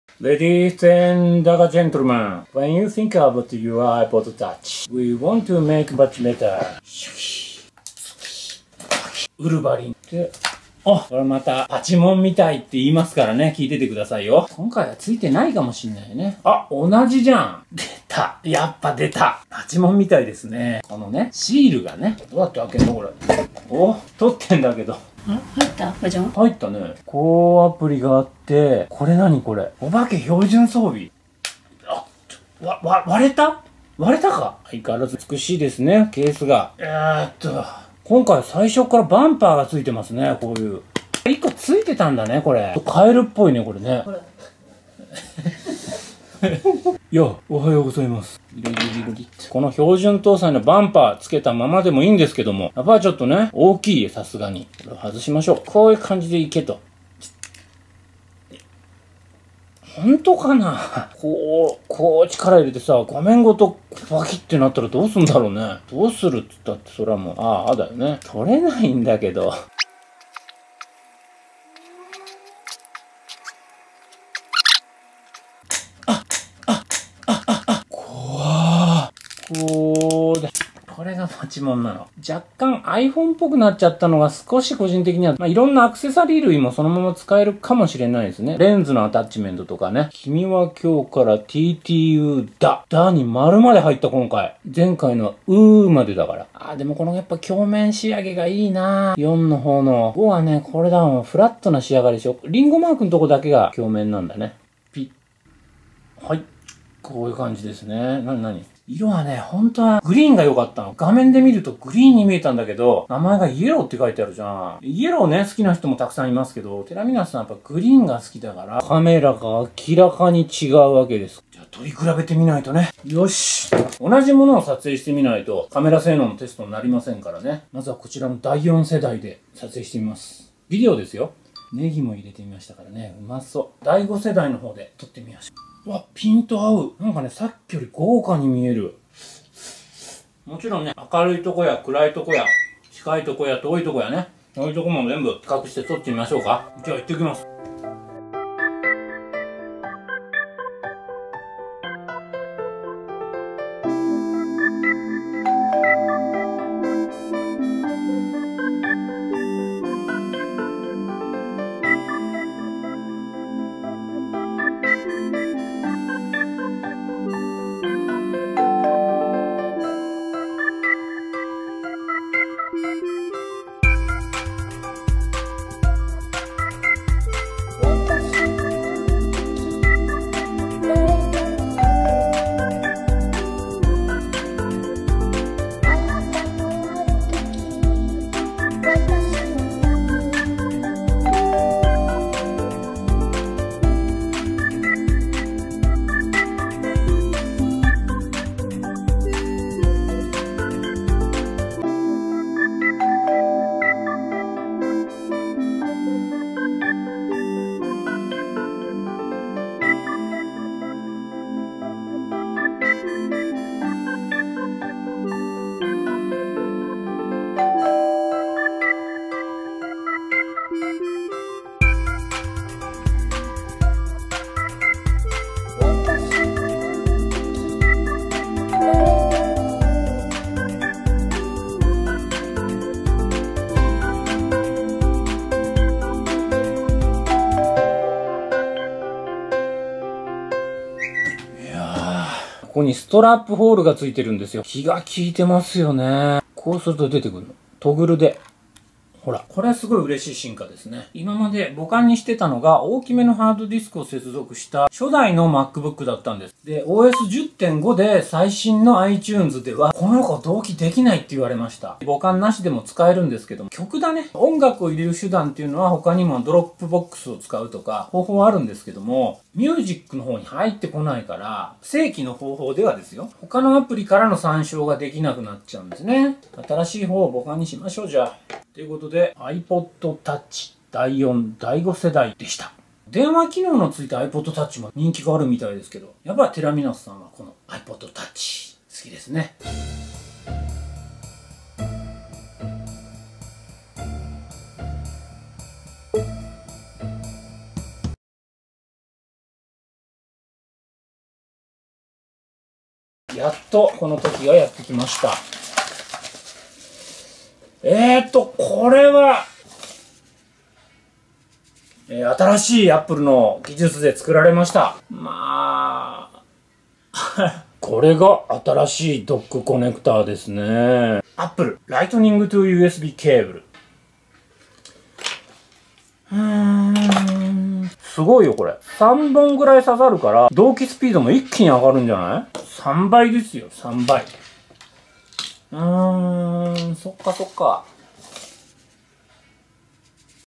比較動画 iPod Touch5 のカメラ性能 / Compared! iPod Touch5 cam, Touch4 cam.
薄い軽い長時間稼働それはiPod Touch. I was captured by the camera. Shooting the same subject for comparison.